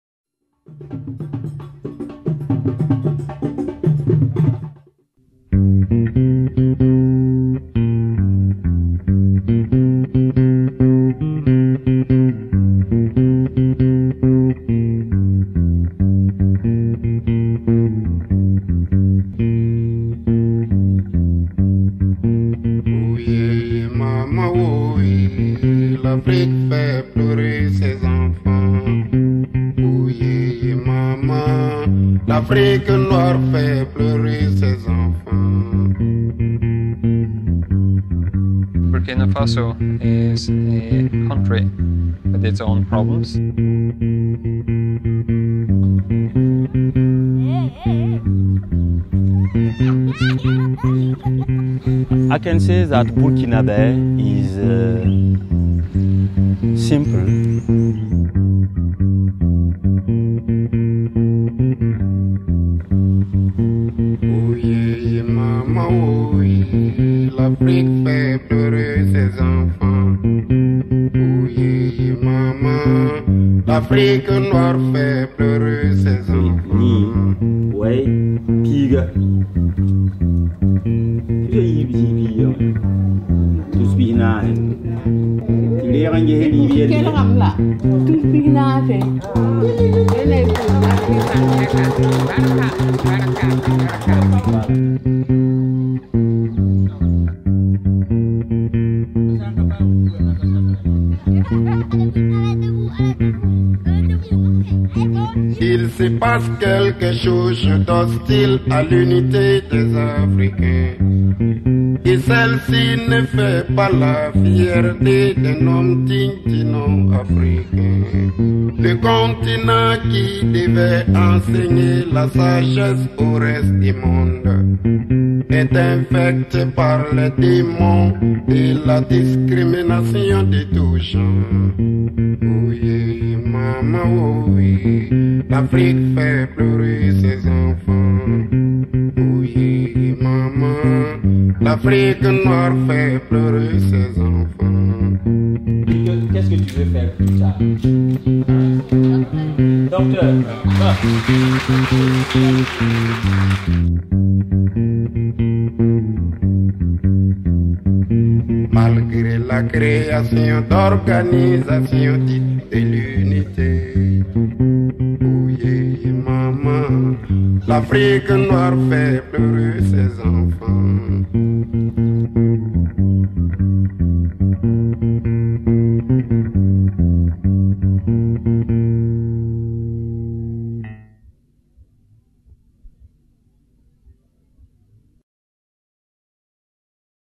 Burkina Faso - A Short Film
A short film inspired from our travels to Burkina Faso, Africa while filming documentaries for A Better Life Foundation.

Directed, shot, and edited by Jacopo Campaiola for SmashWerks Films.